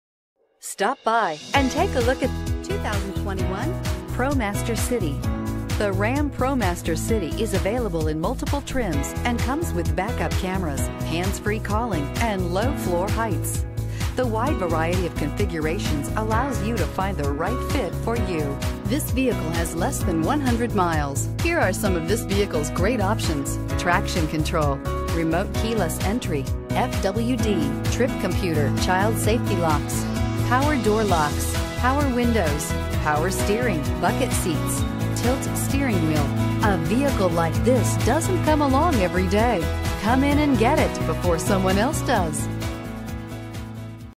2021 Ram ProMaster City Cargo Van Lawrenceville GA L149040
2021 Ram ProMaster City Cargo Van Tradesman

Must print ad and present ad at time of sale to receive quoted price.brbrFactory MSRP: $28,370 $1,067 off MSRP! 2021 Ram ProMaster City Tradesman FWD 9-Speed 948TE Automatic 2.4L I4 MultiAir Bright White